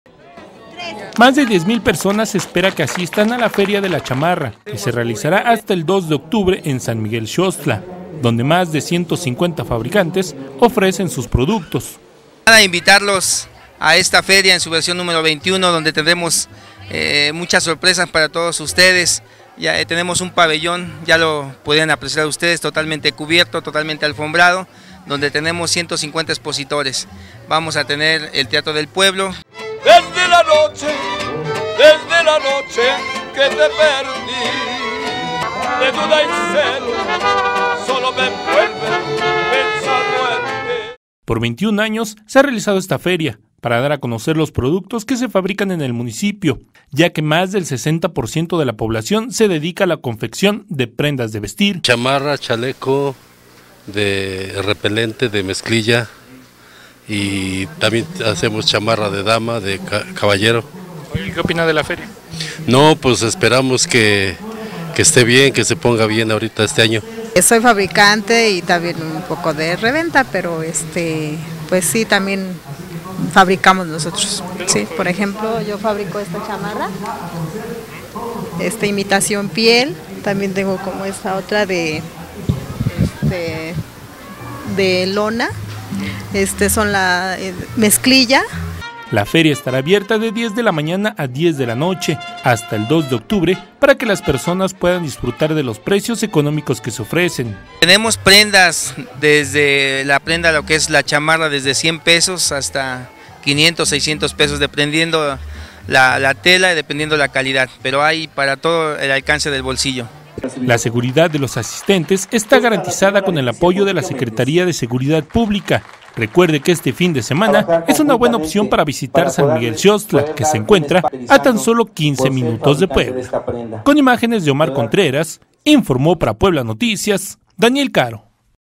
Inicia la expo feria de la chamarra 2011
Se reunirán 150 productores para ofertar diversidad de prendas por diez días